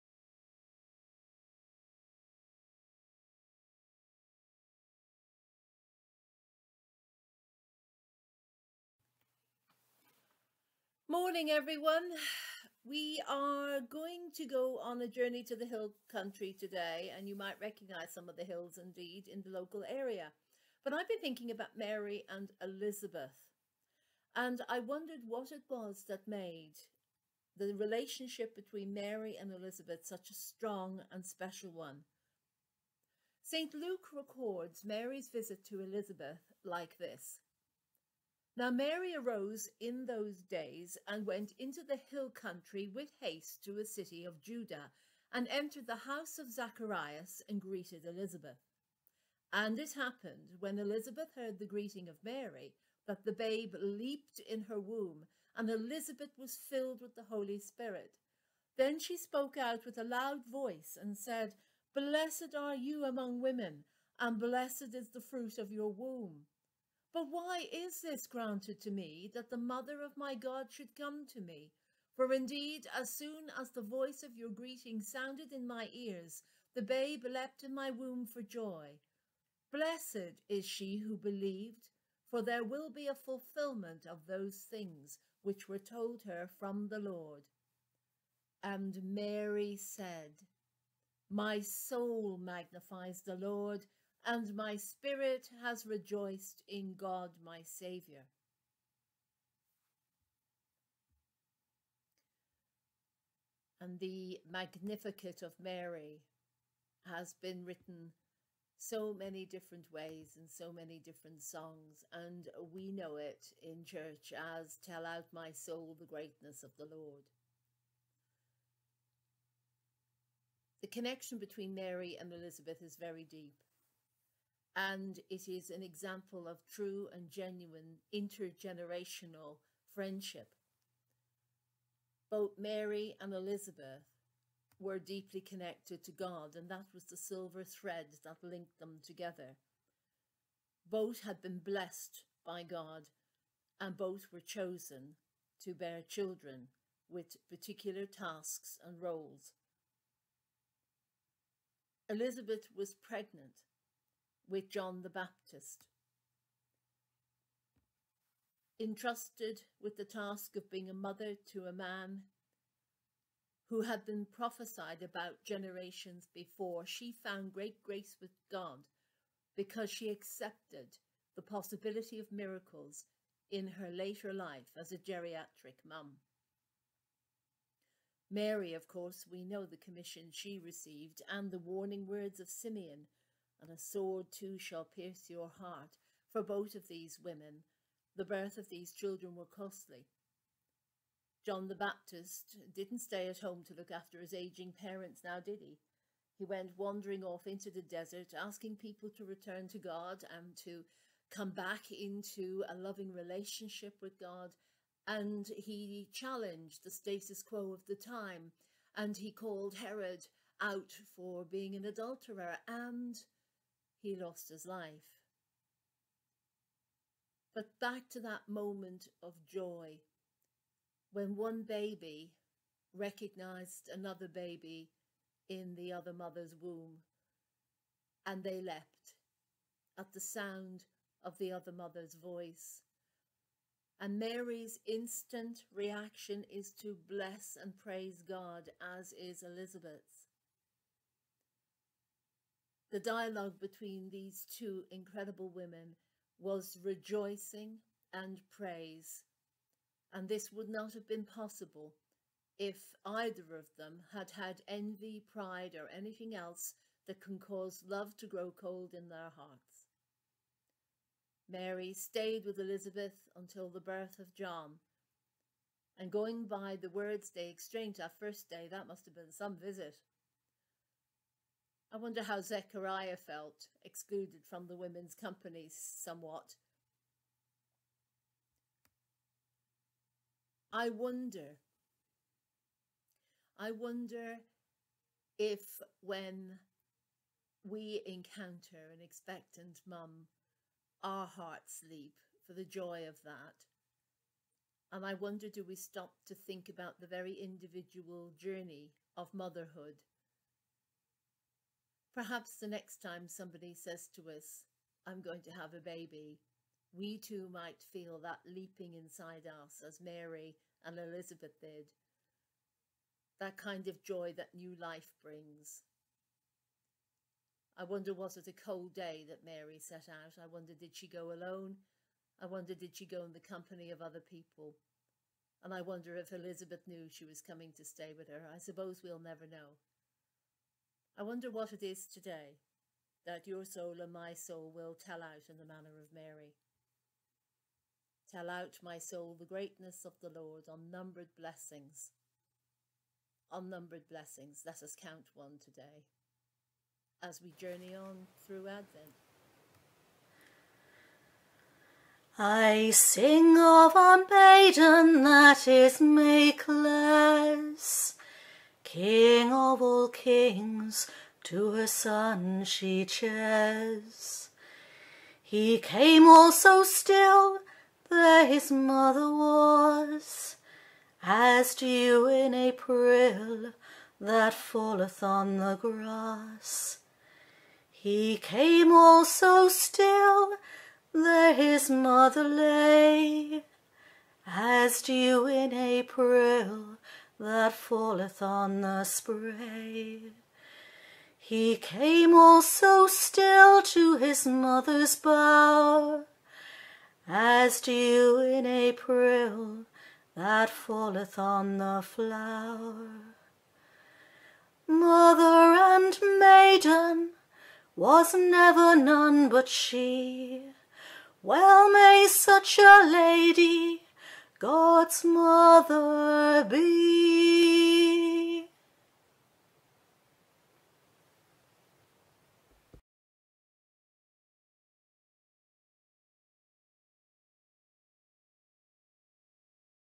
Advent Wanderings Day 9- Mary and Elizabeth
HAve you ever wondered about the relationship between Mary and the elderly geriatric Mum , Elizabeth? ,,,,,,,,Tune in and Listen with Mothers!